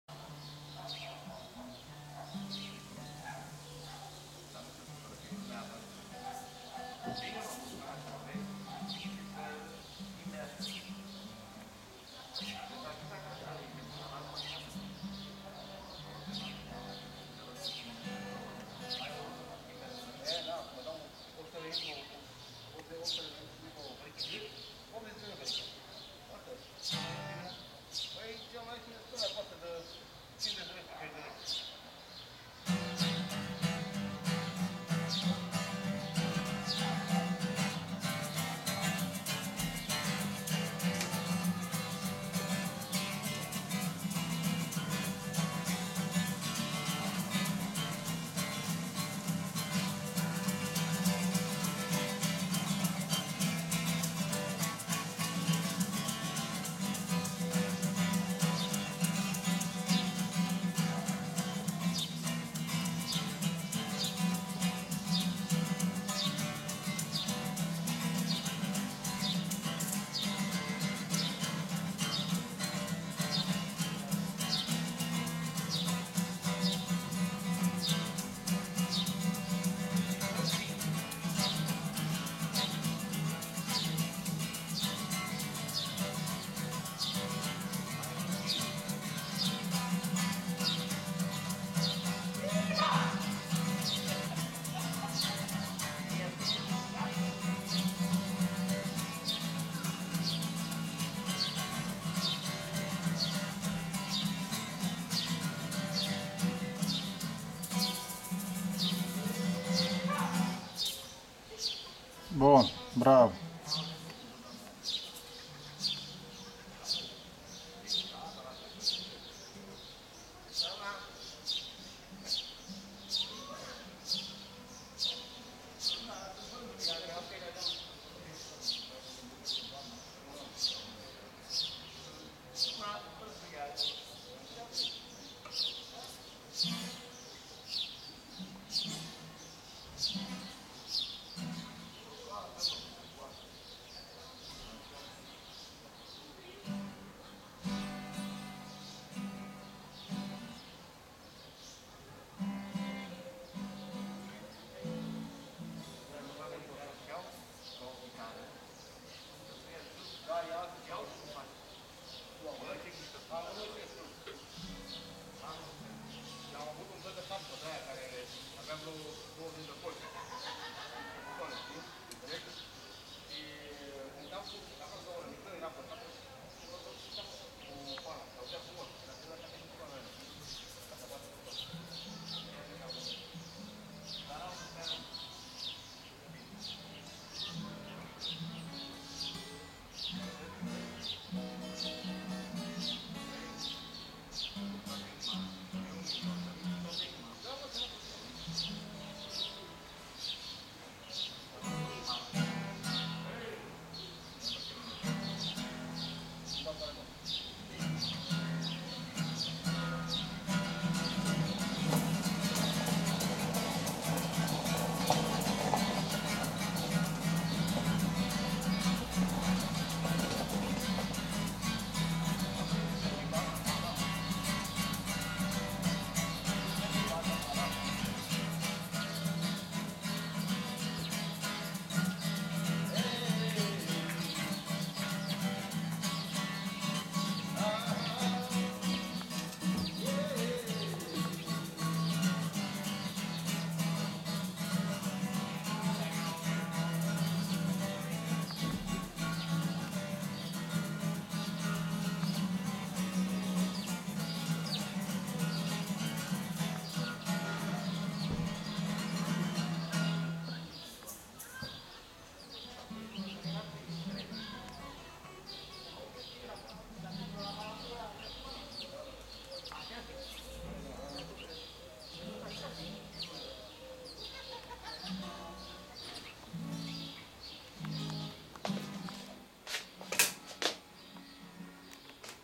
Concert cu măști în curtea blocului - Curaj.TV
Eveniment ad-hoc organizat de niște vecini talentați, care s-au plictisit să stea închiși în casă de frica covid-ului. I-am aplaudat doar eu